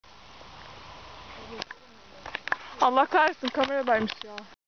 Allah kahretsin kameradaymışım!!!
Fotoğraf çekeceğini sanarken video kaydında olduğunu fark eden kızın dramı... :))